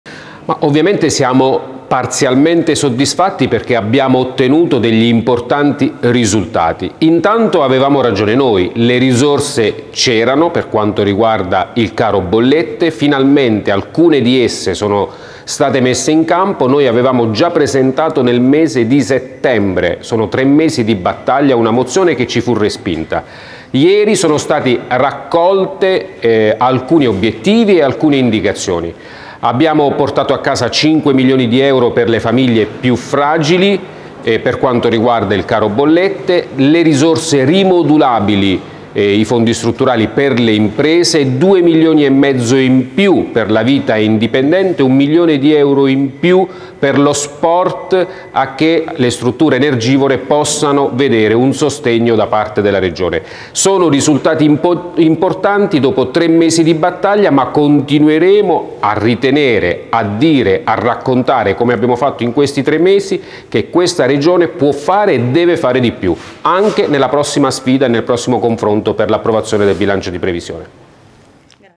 Il resoconto del centrosinistra sui lavori di ieri in Consiglio regionale
I consiglieri regionali del PD Silvio Paolucci e Antonio Blasioli nel corso di una conferenza stampa hanno tracciato un resoconto dei lavori del Consiglio regionale dell'Abruzzo di ieri.